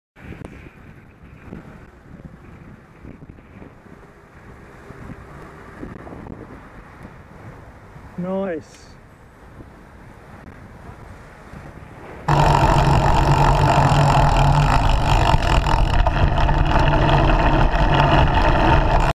Door Zone Danger 31st May 2013
Always got to be careful going down the inside of stationary cars. I was in no danger from this incident due to its timing and my conservative riding. Normally I'd go down the right of that car but the scooter was in the way and I was turning left up ahead.